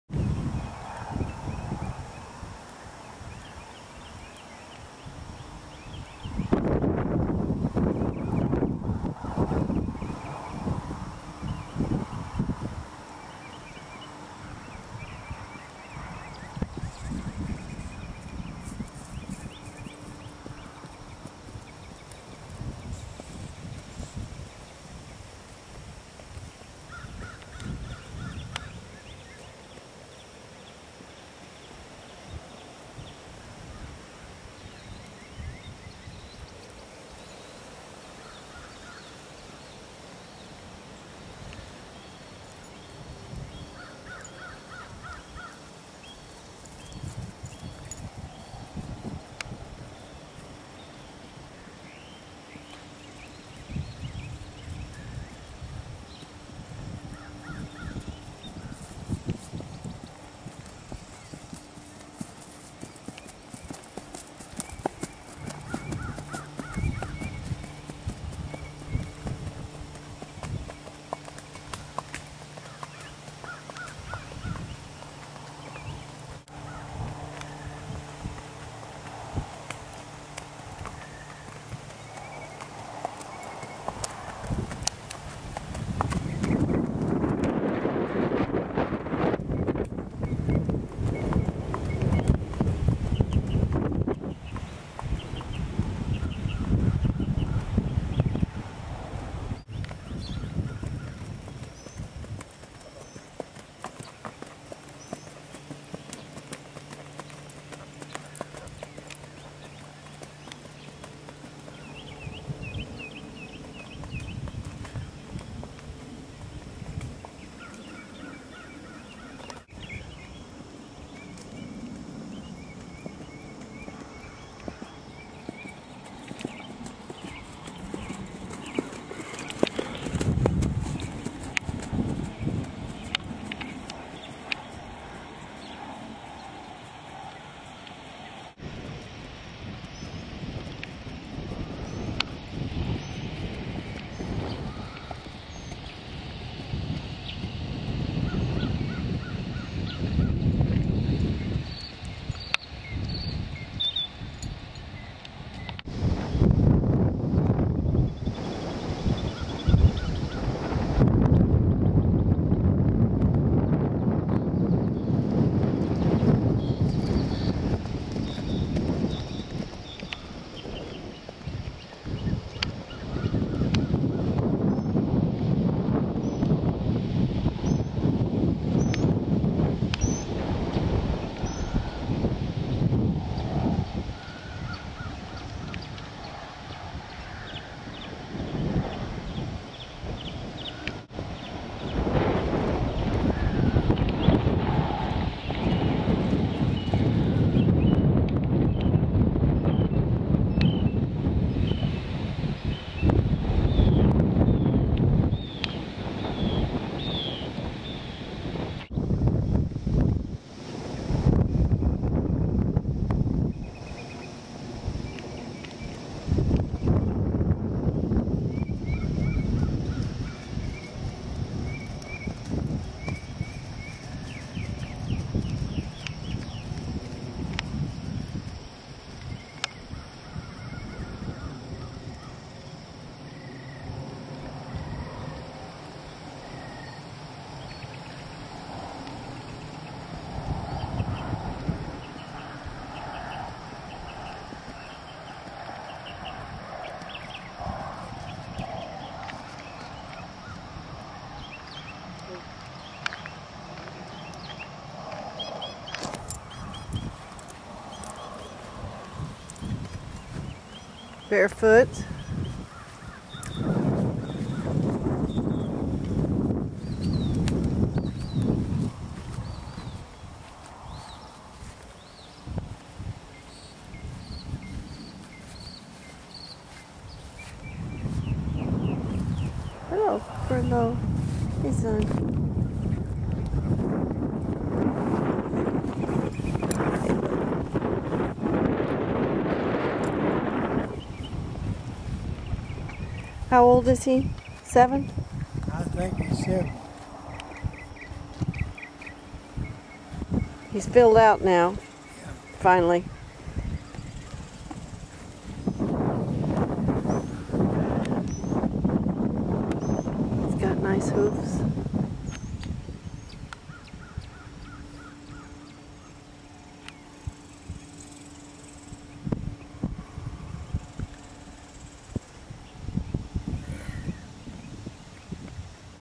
I'm On Furlough
Tennessee Walking Horse, gelding, 8 years old, 15.2 hands.  Nice smooth gait including canter; willing disposition; does great on trails, but has mostly been ridden here on the property.  Furlough has a friendly personality and owners have reluctantly decided to sell due to health issues.  Ridden barefoot and in a curb bit in this video, but does just as well in a snaffle.